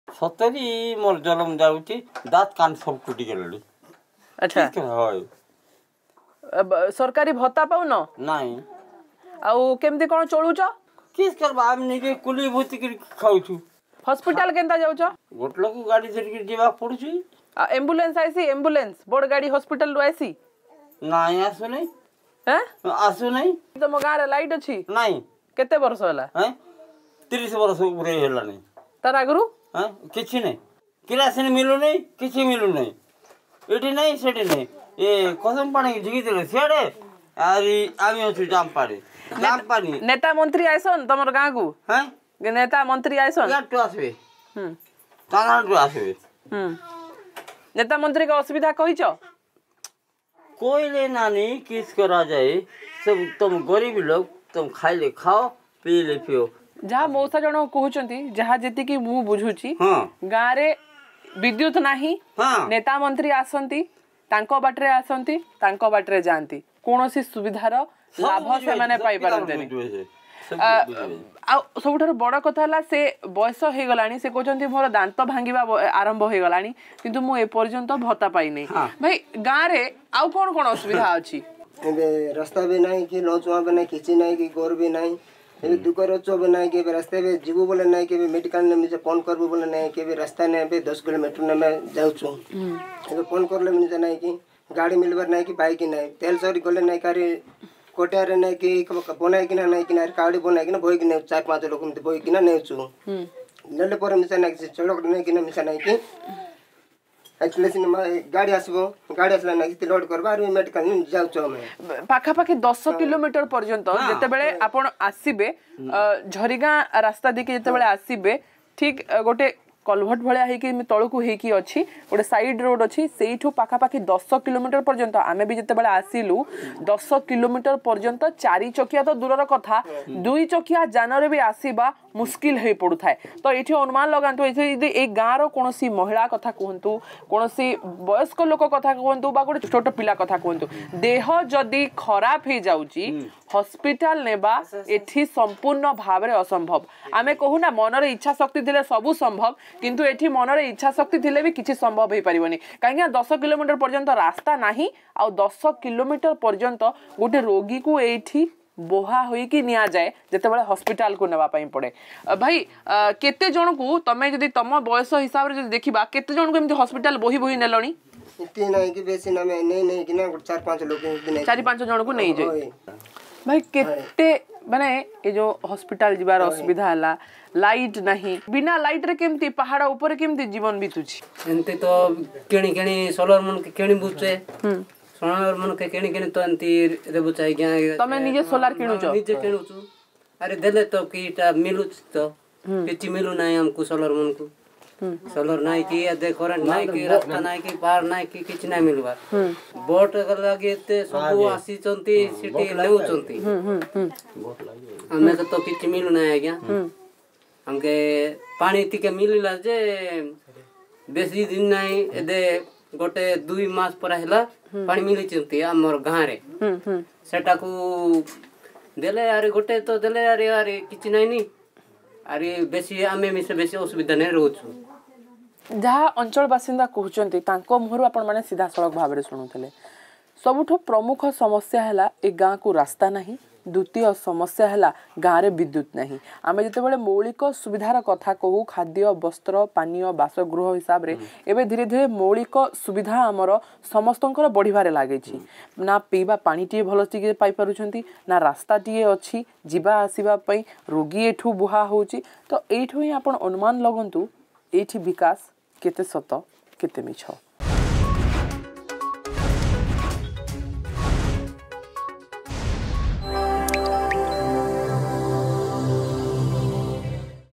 Nabarangpur: Road and Basic Facilities Still Elude This Remote Village
Road and Basic Facilities Still Elude This Remote Village in Nabarangpur.